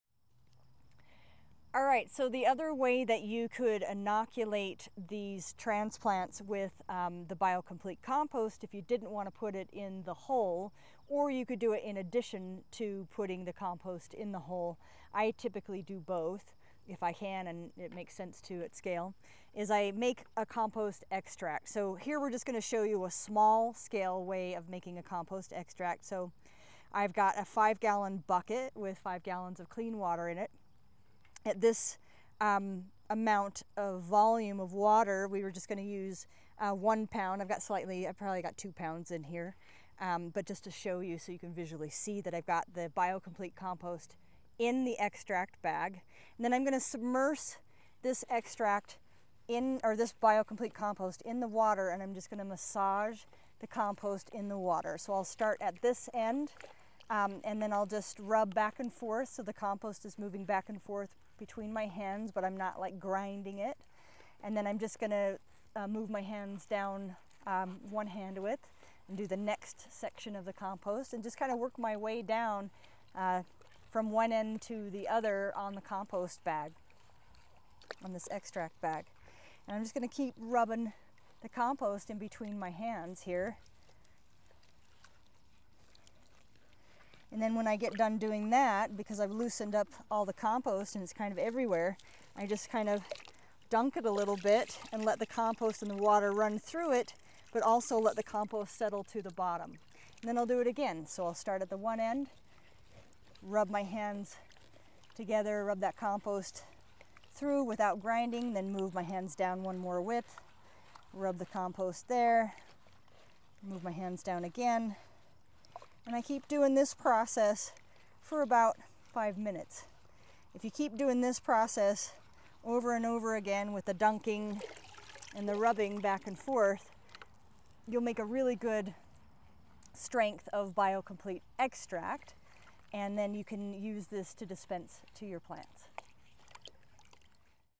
Market Farming Series: How to Make a Compost Extract
Sara describes how to make a compost extract in a video from her new series on Market Farming.